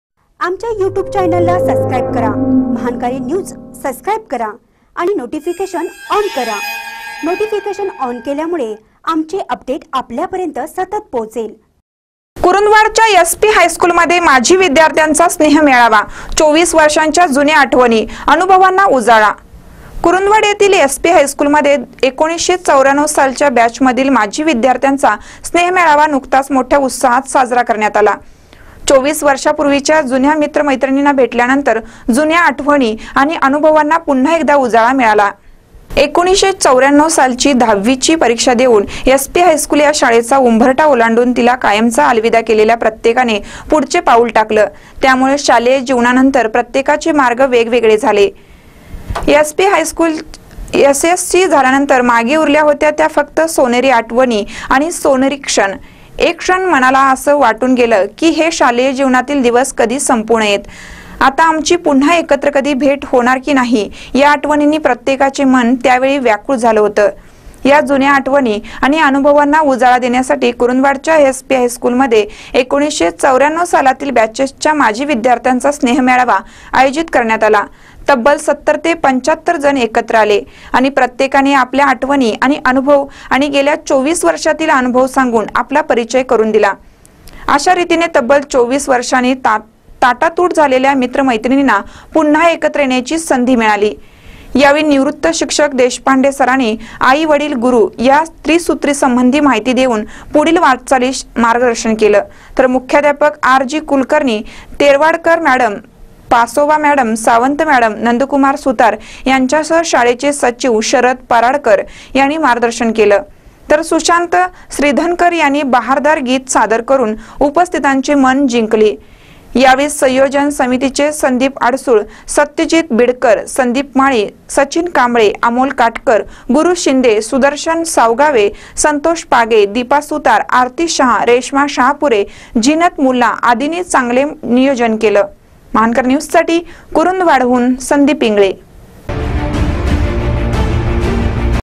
कुरुंदवाडच्या एस पी हायस्कूलमध्ये माजी विद्यार्थ्याचा स्नेहमेळा
कुरुंदवाडच्या एस पी हायस्कूलमध्ये माजी विद्यार्थ्याचा स्नेहमेळा : २४ वर्षानी जुन्या आठवणी, अनुभवांना उजाळा